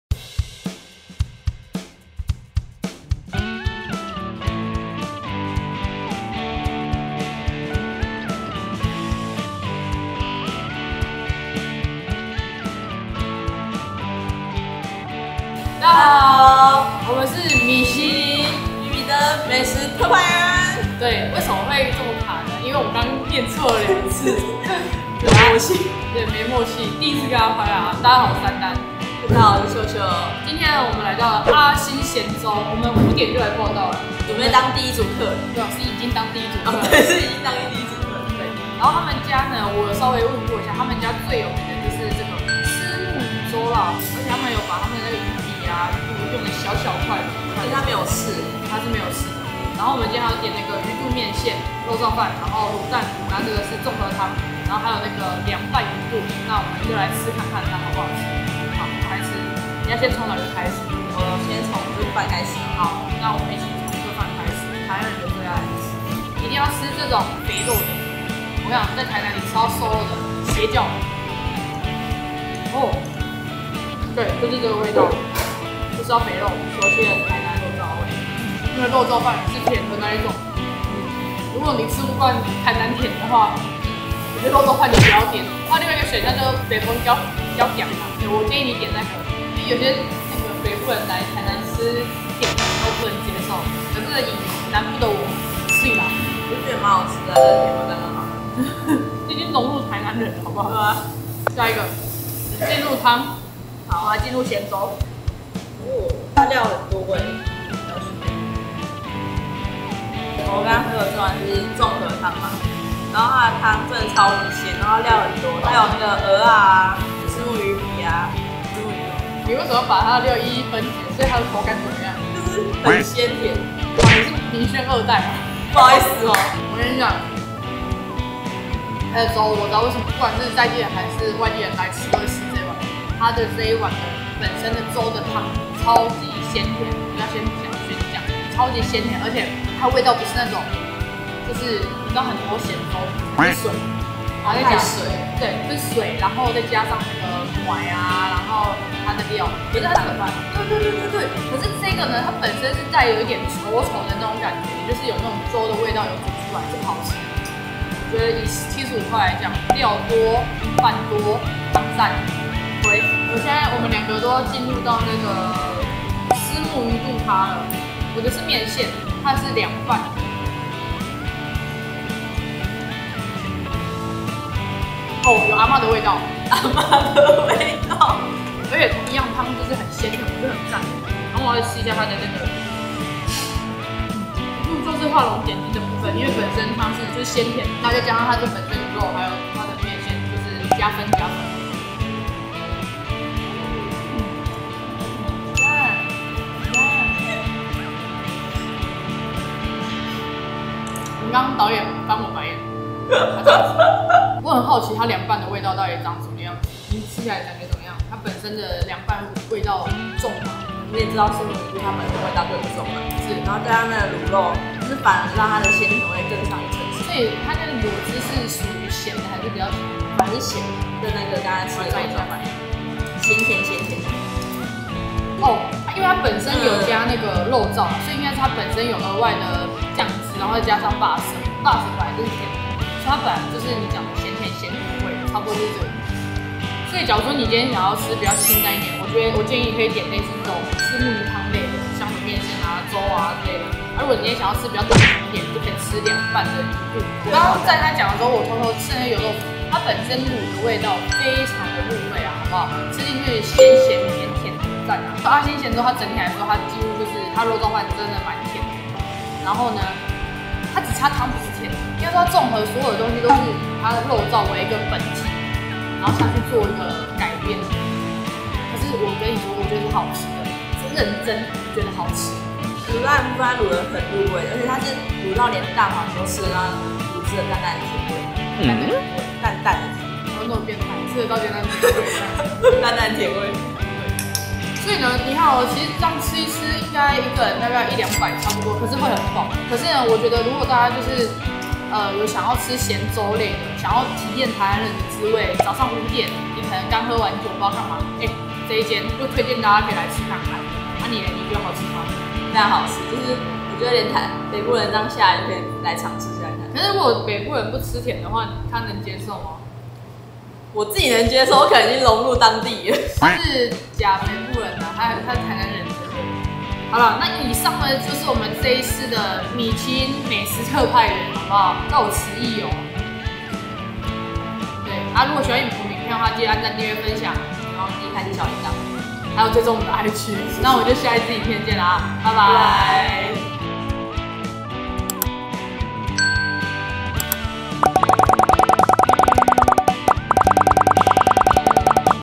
米其林必比登推薦系列｜民族路阿星鹹粥｜55年的歷史 超鮮甜鹹粥每桌必備一碗 台南風味肉燥超讚!!
每週四、每週日更新影片
追蹤俞萱的IG：carine_0728
追蹤丹丹的IG：daven.0315
追蹤團體的IG：ayayanlei